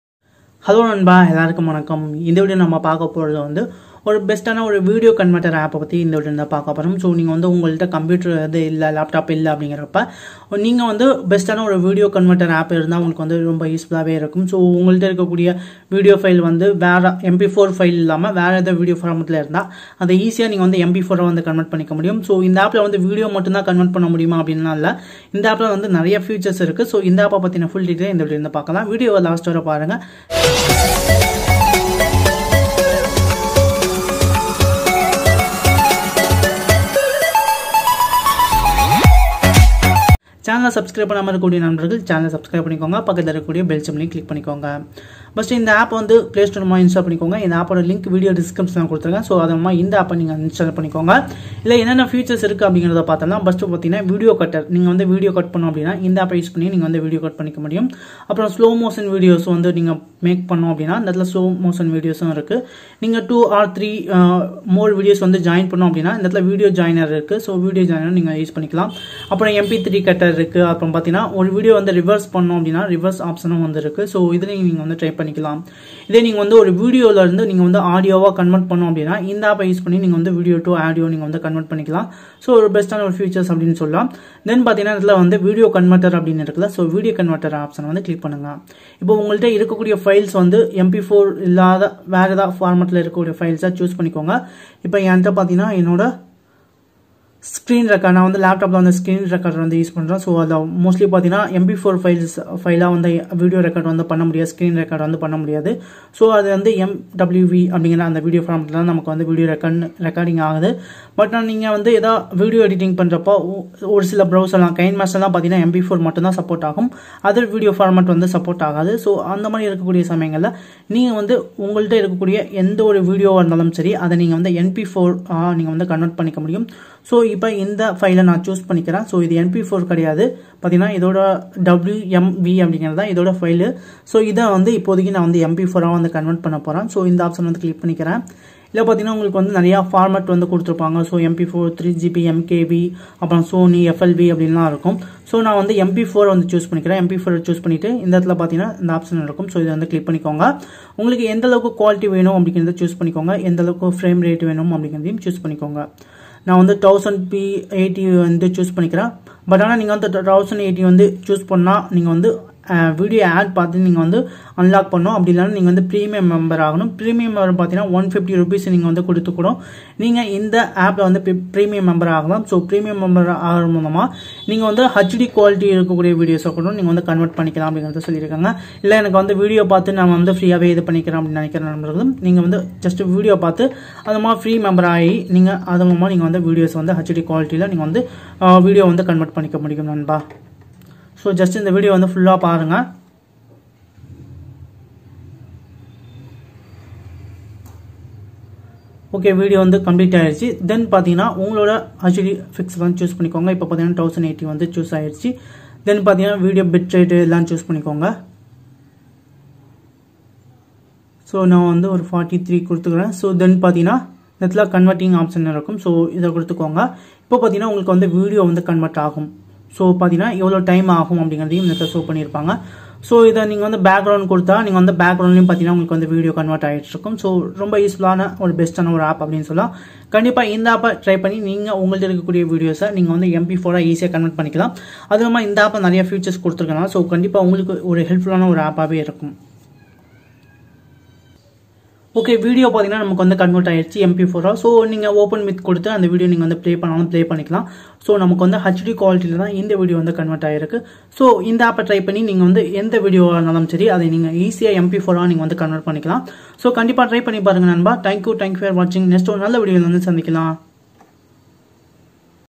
Best Free video converter app for Android phone in Tamil | Prathap Tech
Hi friends, Welcome to Prathap Tech, In this video I'm going to show you the Best Video Converter app for Android Phone, An application has more useful tools like video converter, video compressor, video to audio converter, trim video and audio, slow motion video, reverse video, rotate video, video merging, video cutter and the like.

Title also relate:
video converter app for android
best video converter app for android
video compressor app for android